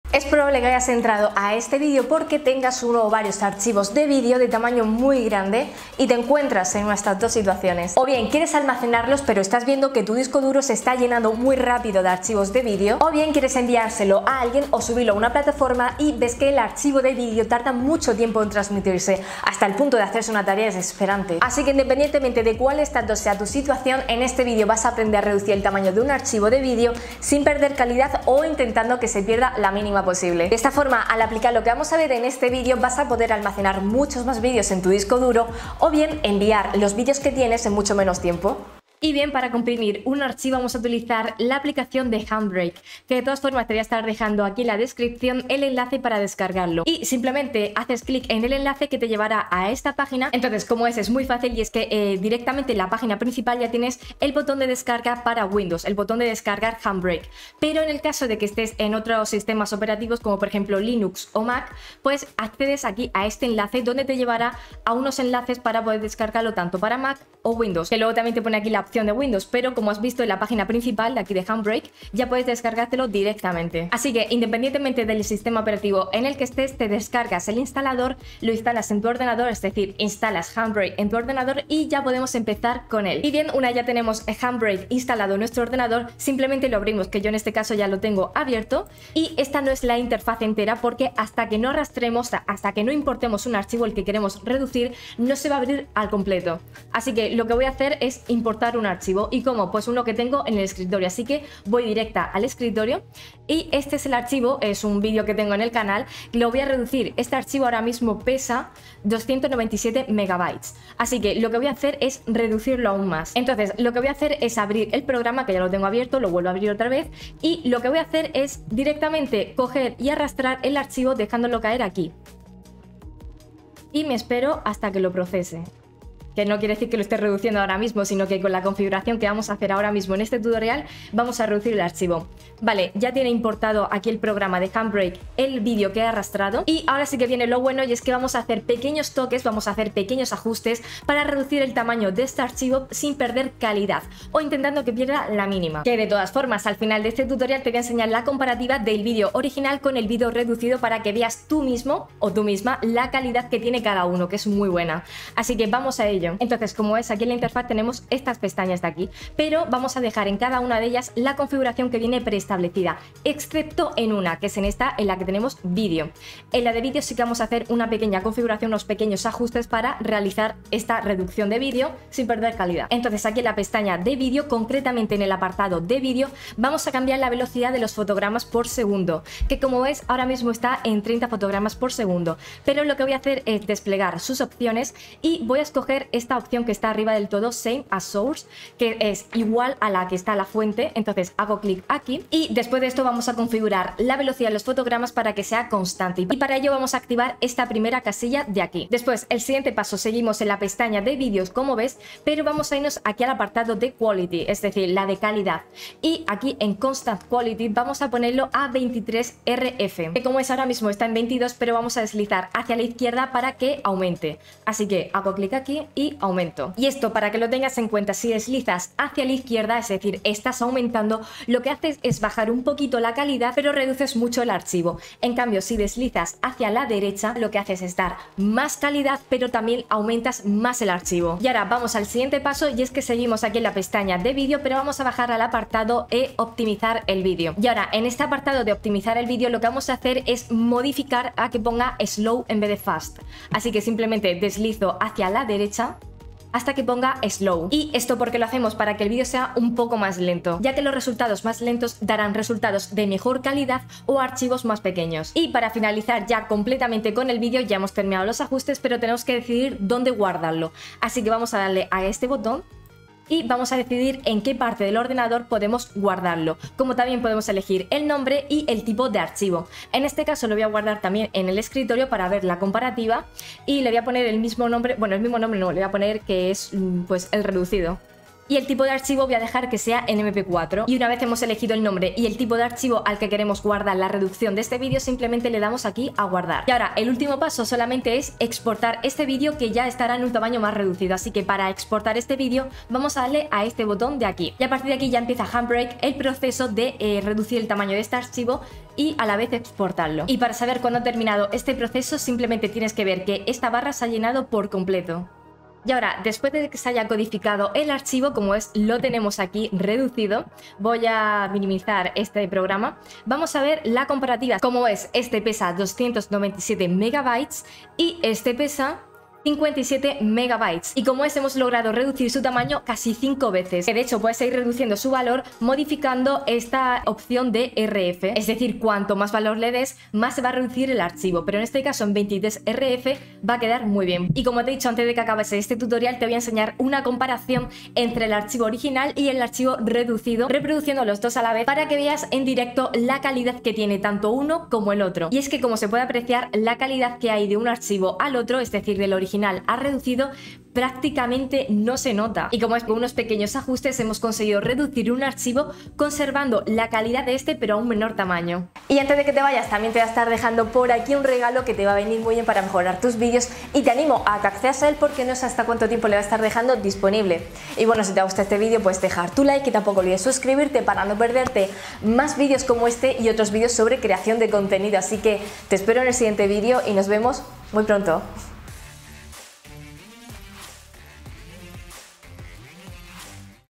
Cómo Comprimir un Archivo de Vídeo Sin Perder Calidad | Reducir el Tamaño de un Archivo de Vídeo
En este vídeo vas a aprender cómo reducir el tamaño de un archivo de vídeo sin perder calidad, o intentando que se pierda la mínima posible. De esta forma, al aplicar lo que vamos a ver en este vídeo vas a poder almacenar muchos más vídeos en tu disco duro o enviar los vídeos que tienes en mucho menos tiempo.

Espero que te haya sido de gran ayuda y... ¡Un abrazo! :)

-Marina